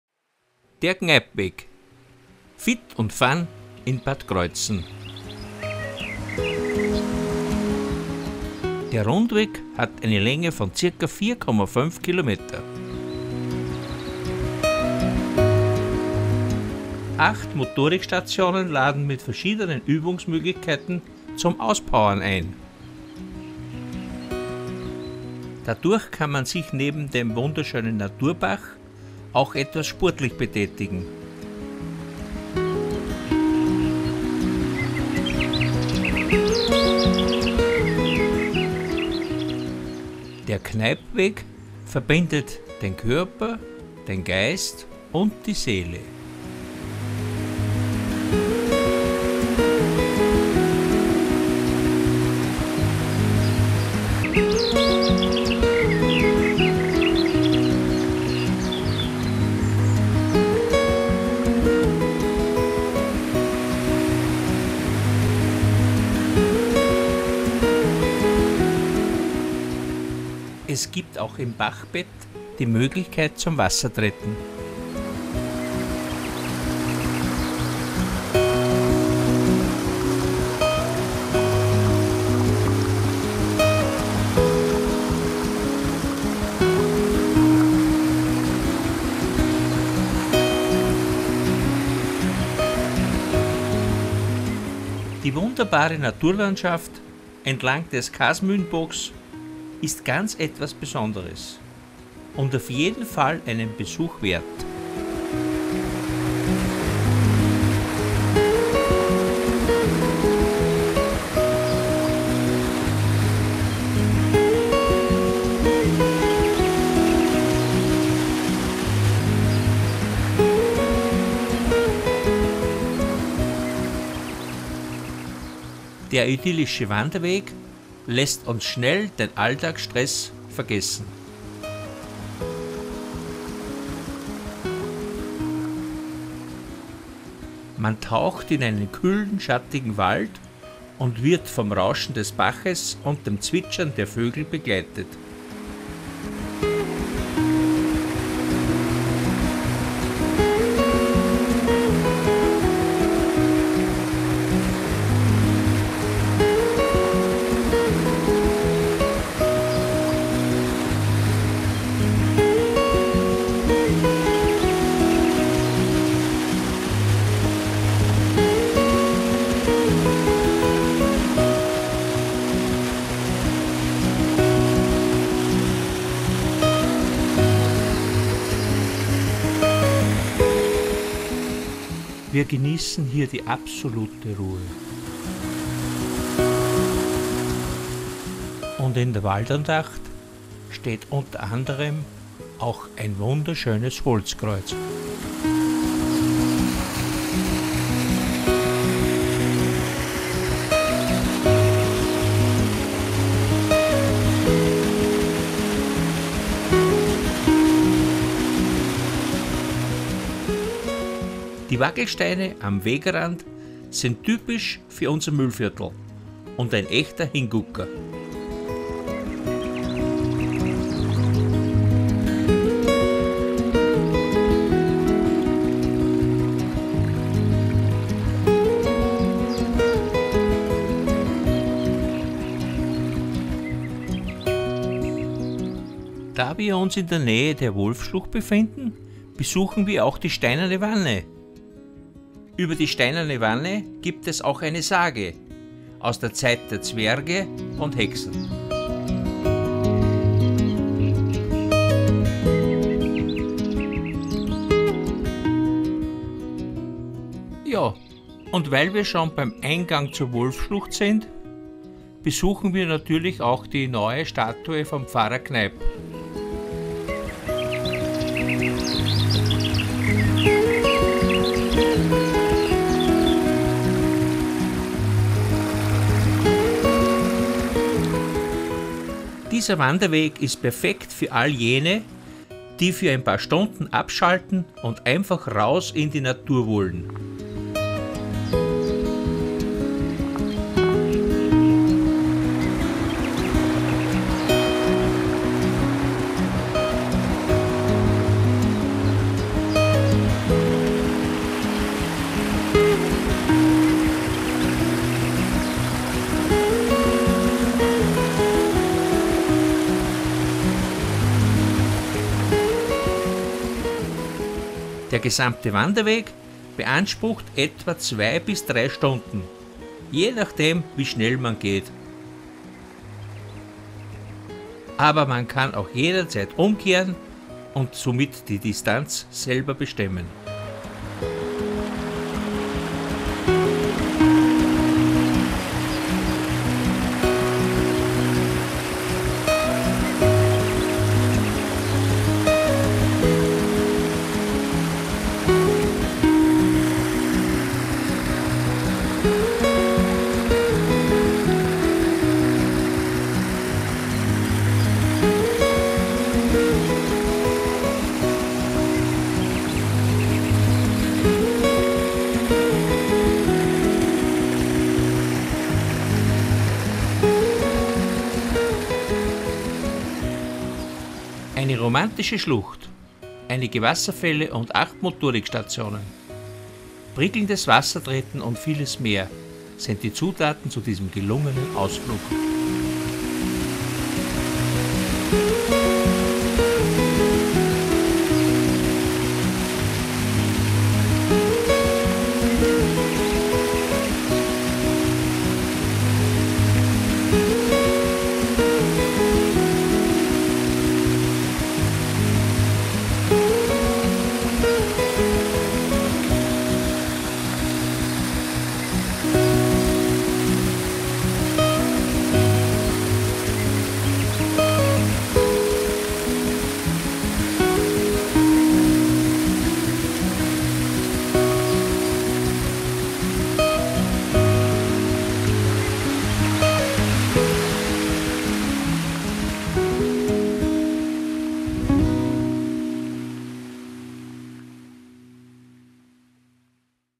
Kneippweg | Bad Kreuzen | fit & fun
AUSZEIT am Kneipp Weg in Bad Kreuzen. 8 Motorik-Stationen laden mit
verschiedenen Übungsmöglichkeiten zum Auspowern ein.